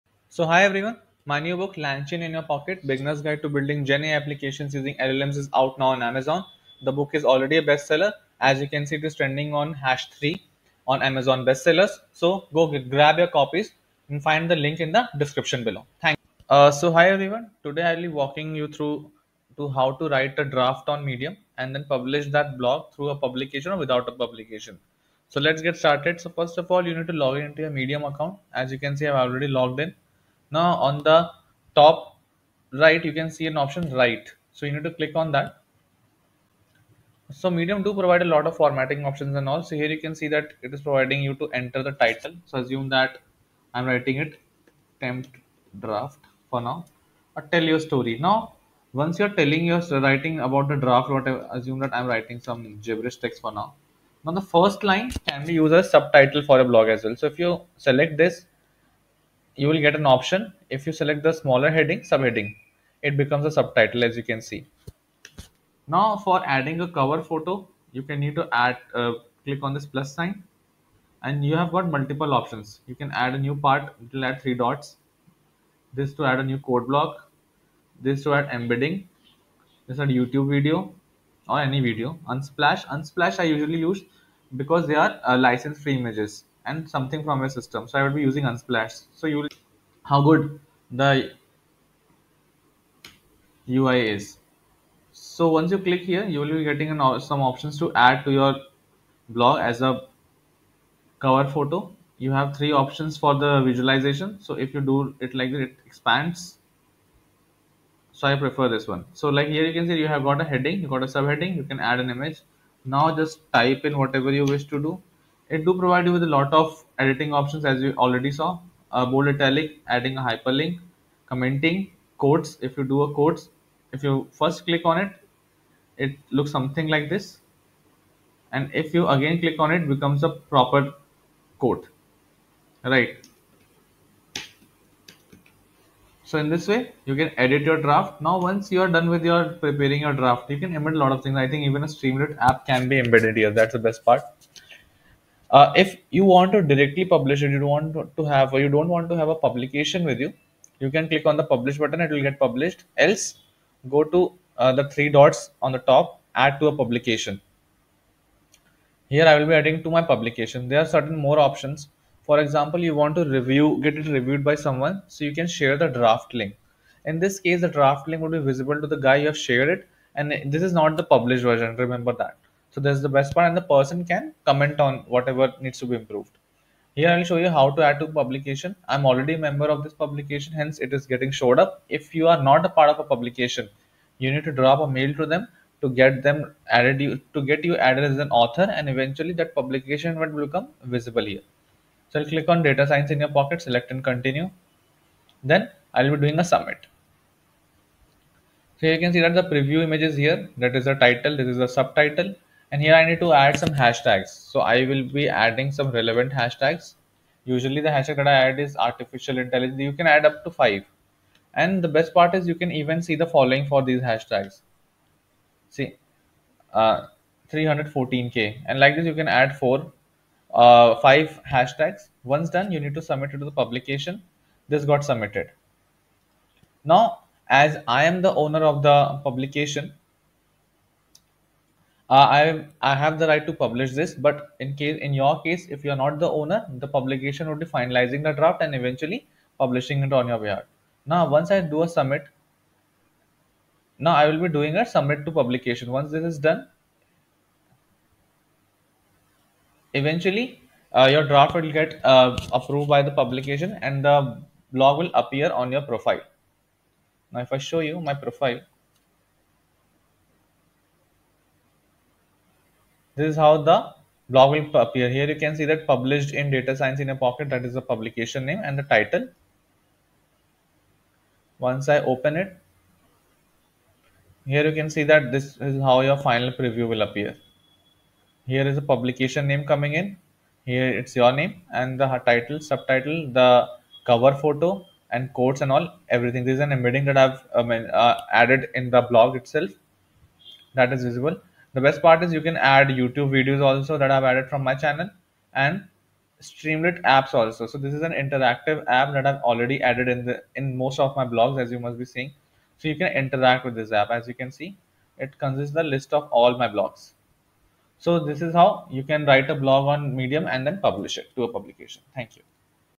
How to publish blogs on Medium?
This tutorial is a step by step walkthrough on how to write a draft and publish a blog on Medium, the blogging website

LangChain in your Pocket: Beginner's Guide to Building Generative AI Applications using LLMs